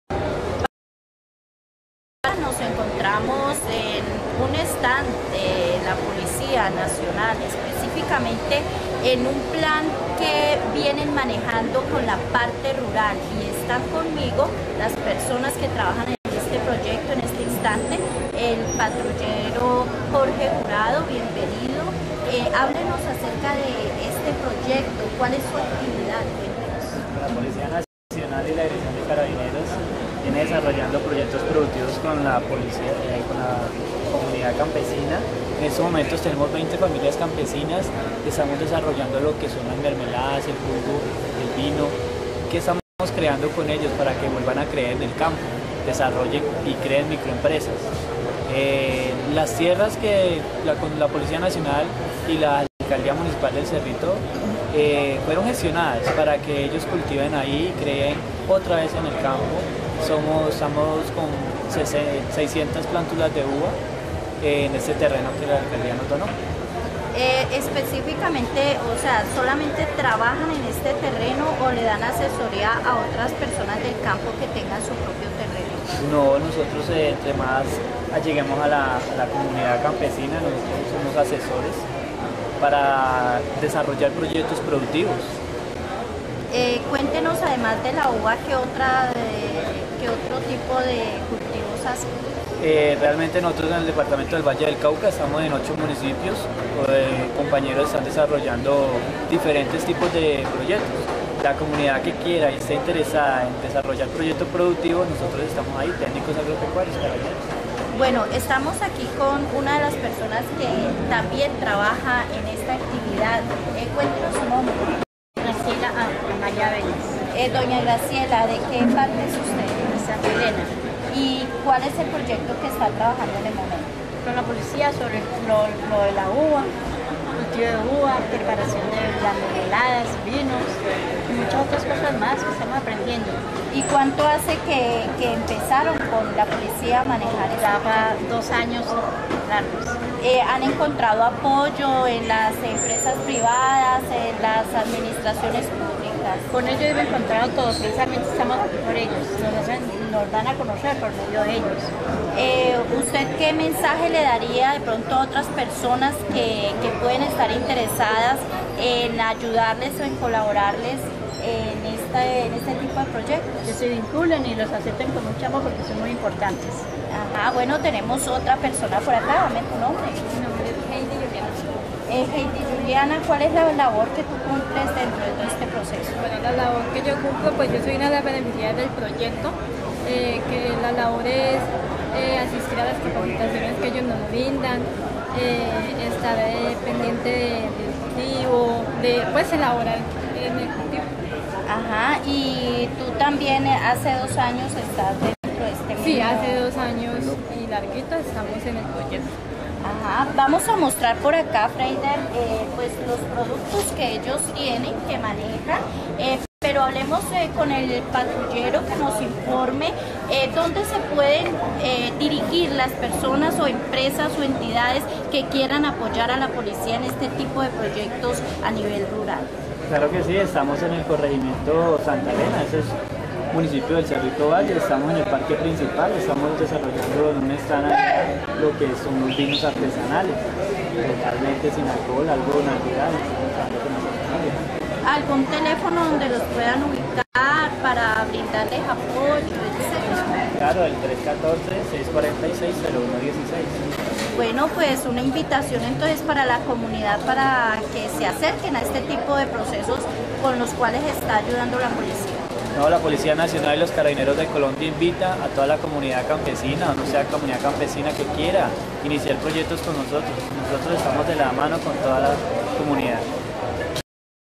COLISEO DE FERIAS: STAND PROMOCIONA PRODUCTOS DE UVA ZONA RURAL APOYADA POR LA POLICIA
PALMITVEOCOLOMBIA
Gerente Adriana Restrepo
Apoyo Periodístico: Freyder Hernandez
COLISEO DE FERIAS: STAND PROMOCIONA PRODUCTOS DE UVA  ZONA RURAL APOYADA POR INGENIEROS DE  LA POLICÍA
Noticias de Palmira para el mundo a un solo y